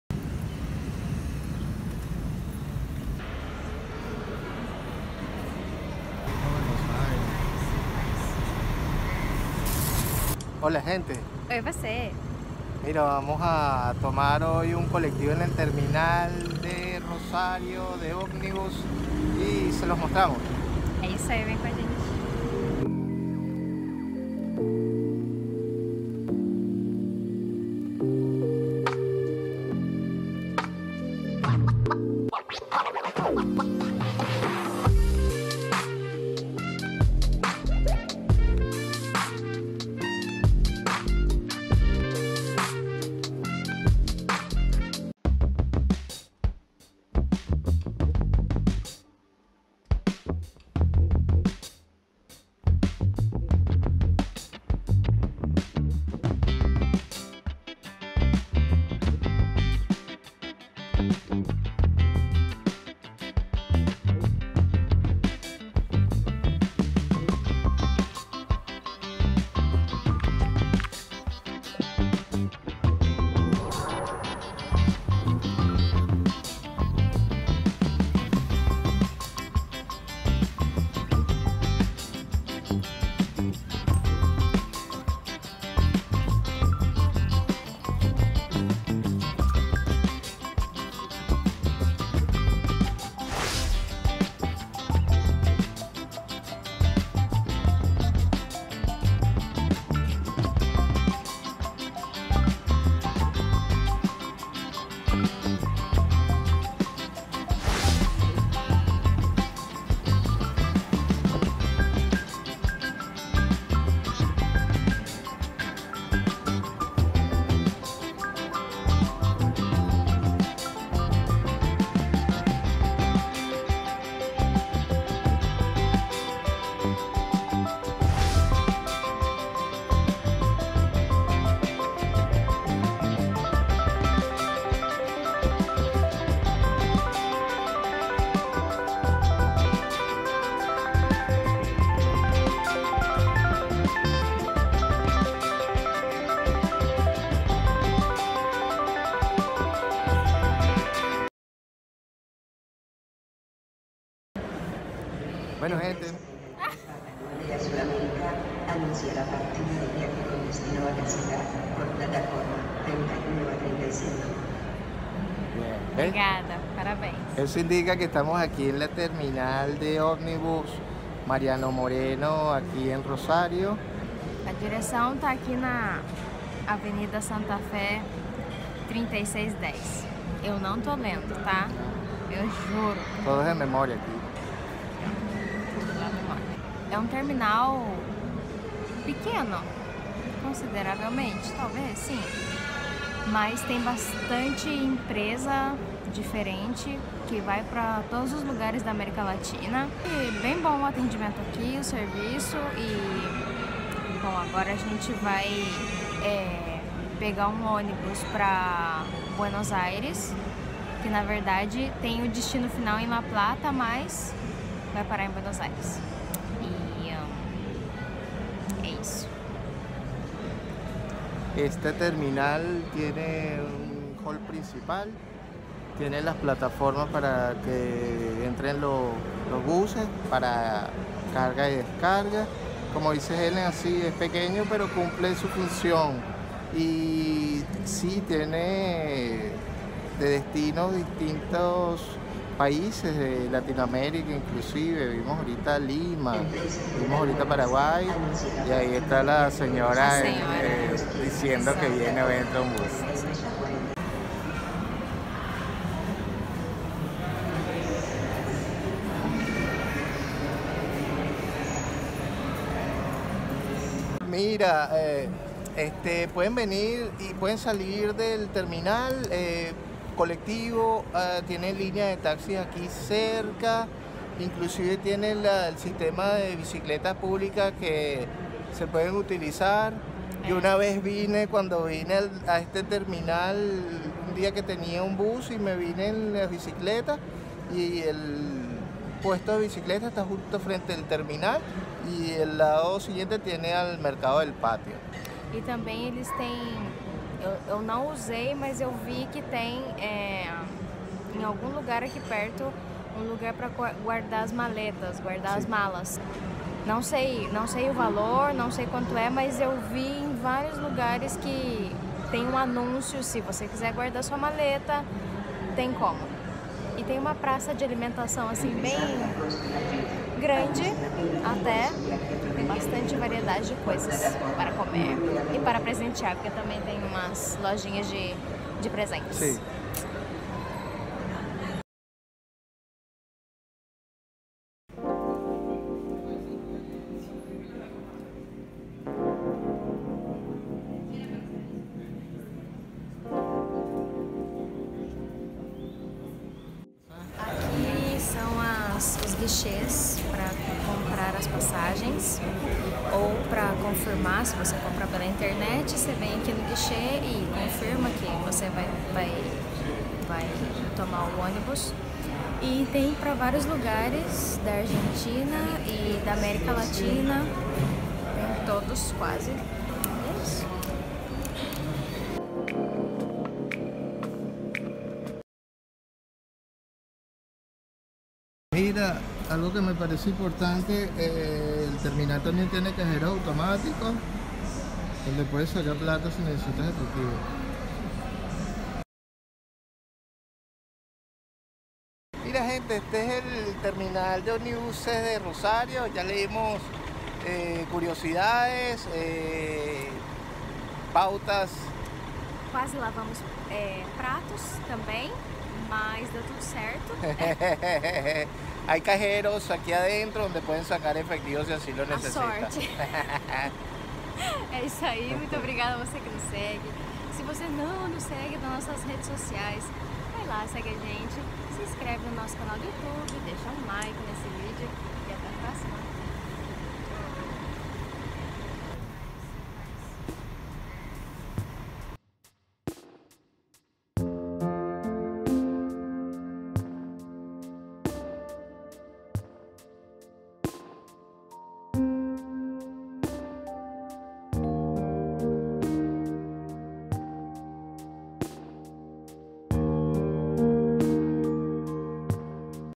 Estación Terminal de Ómnibus Mariano Moreno 🚌 | ROSARIO, ARGENTINA 🇦🇷 | VLOG VIAJANTE 🌎
¡Hola gente! Ya que teniamos que tomar un ómnibus de Rosario a Buenos Aires desde la Estación Terminal de Ómnibus Mariano Moreno, aprovechamos la oportunidad, documentamos y se los mostramos. Esperamos que les guste y les sirva.

00:00 Intro
00:45 Terminal de Ómnibus Mariano Moreno
06:31 Resumen
06:54 Despedida
07:12 Par de Sorrisos

Esperamos que les guste, lo hicimos con mucho cariño.

Sé feliz y bebe agua 💧

¡Un beso! 💙

Par de Sorrisos es la visión de Wilfredo Chiquito y Helen Negrisoli de nuestras aventuras alrededor del mundo. ¡Únetenos!